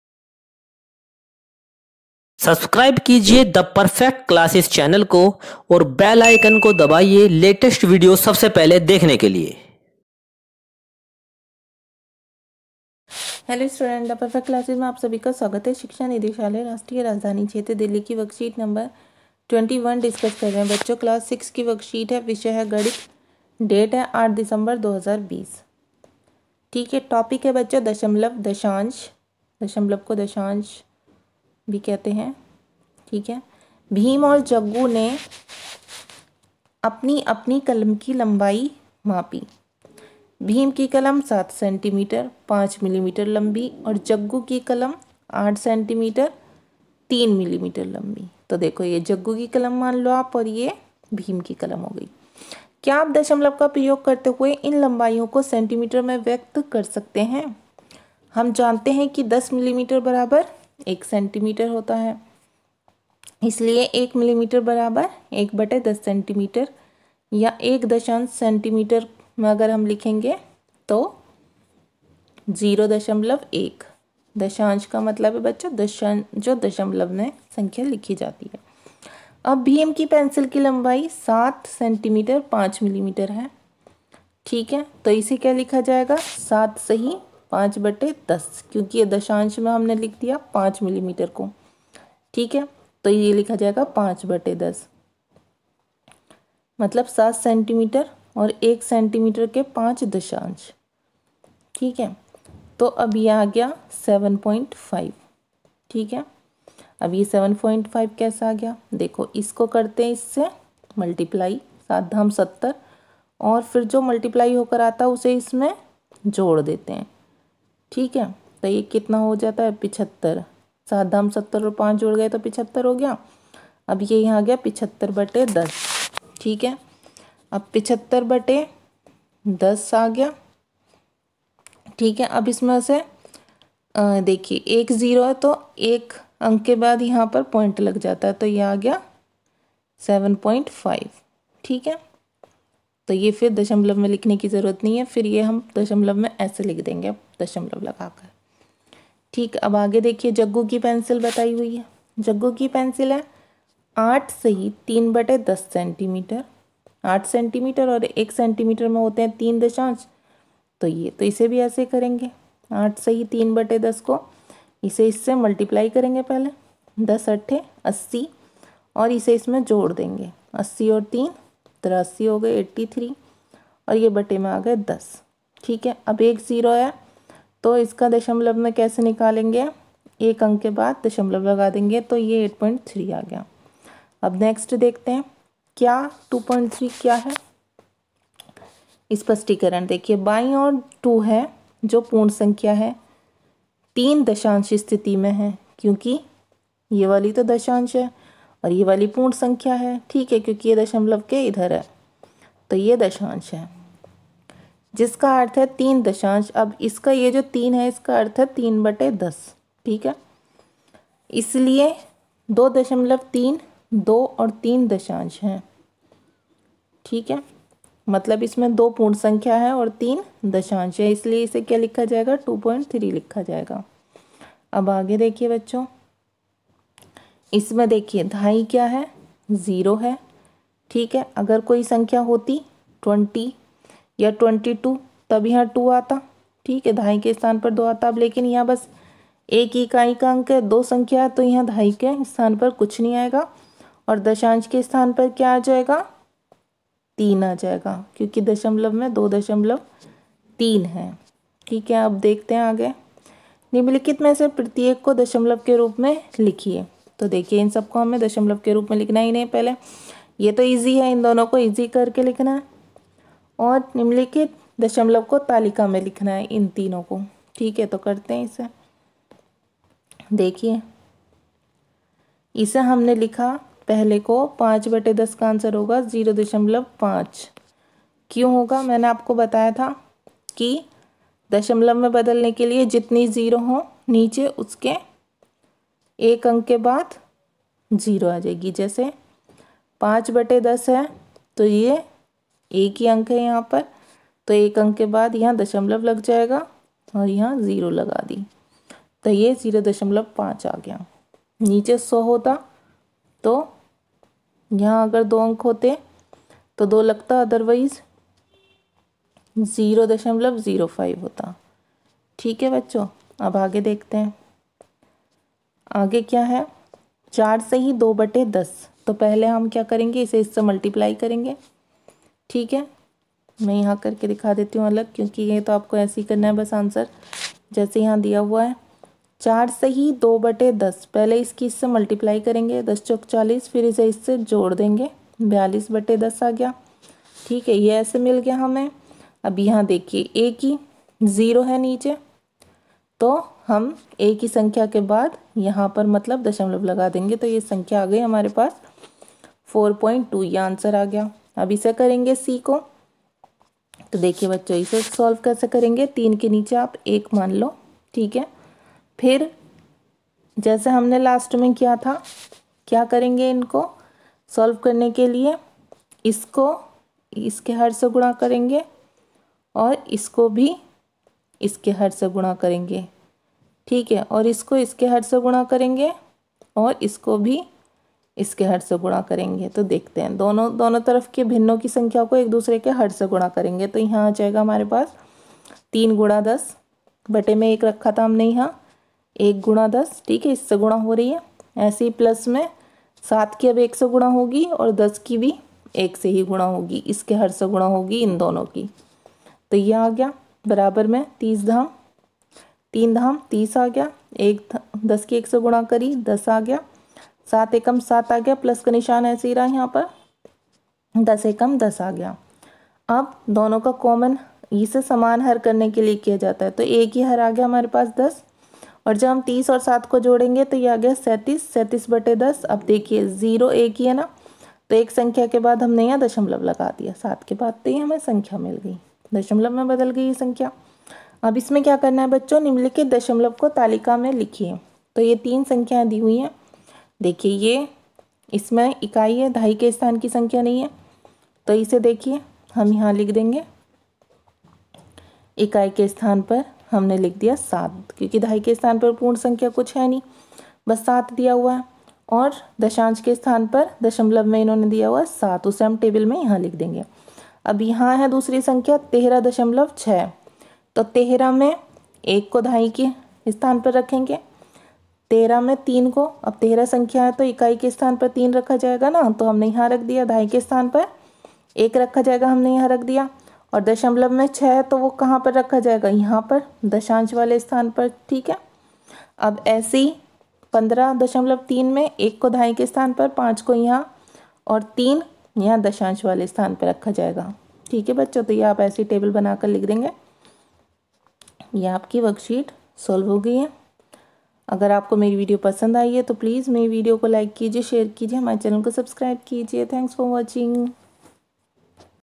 DOE DELHI WORKSHEET 21 CLASS 6( MATHS) HINDI MEDIUM TUESDAY 8 DECEMBER 2020
maine is video ke madhiyam se DOE, DELHI  WORKSHEET 21 FOR CLASS 6 DECEMBER  2020 KO SAMJHAYA HAI..